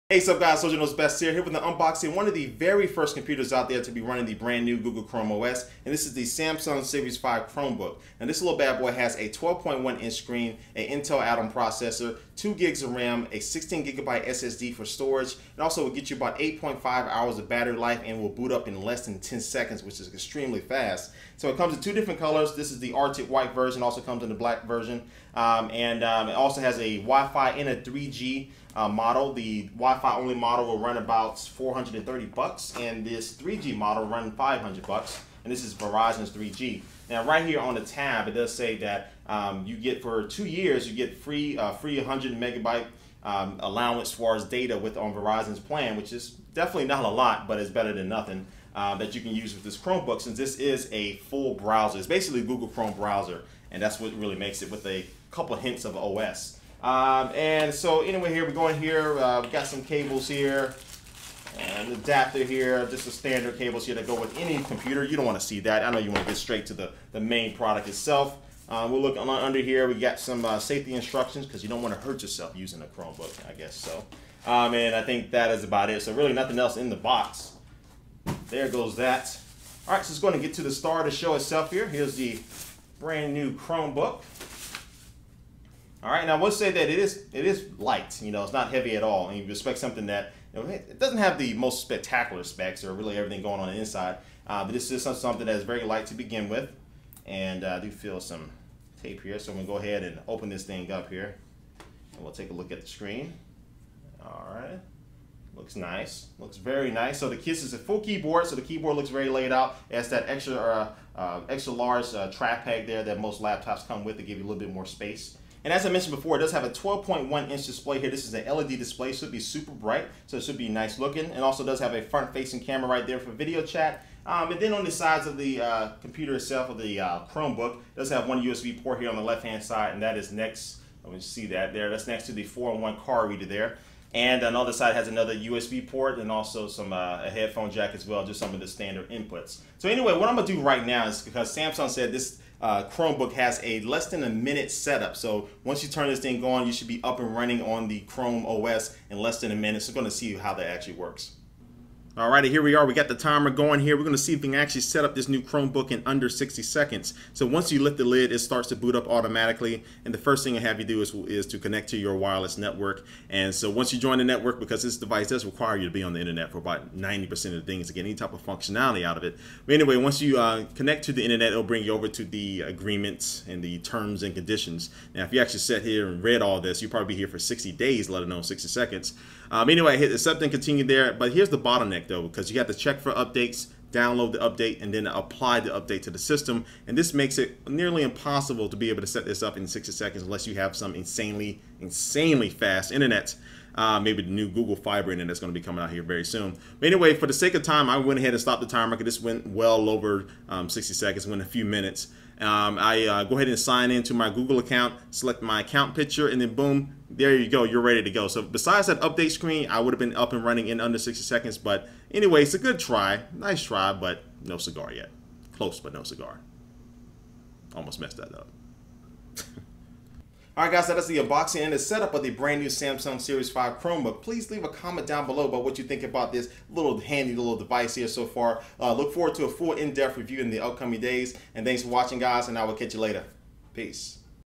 Soldier Knows Best - Unboxing: New Samsung Google Chromebook
In this video, I unbox and setup the new Samsung Google Chromebook.